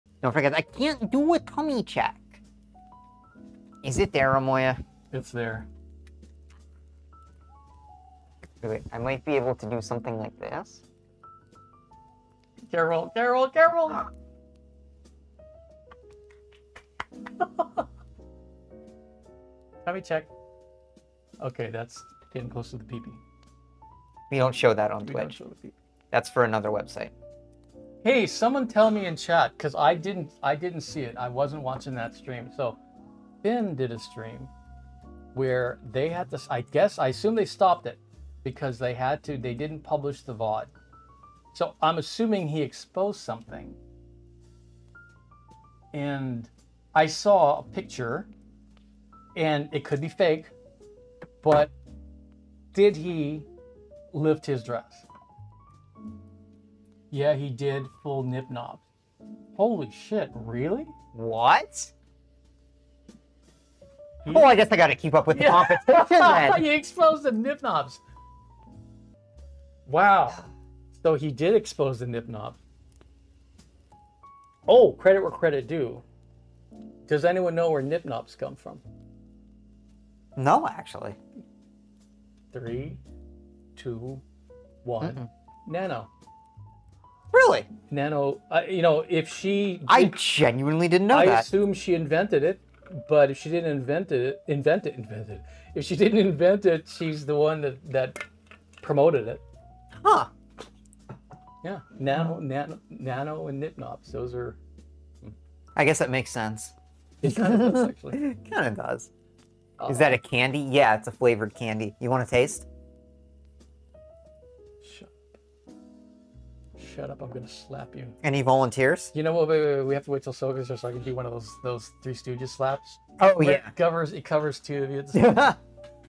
Tummy check and F1nn's nip nop reveal wut
This is a clip from FemboyDrumming's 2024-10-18 Twitch stream entitled A Maddie and Moy Moy Stream!｜ !socials 🥵

Clipped after the stream.

This is a fan-made and uploaded video. FemboyDrumming retains all rights to the stream content.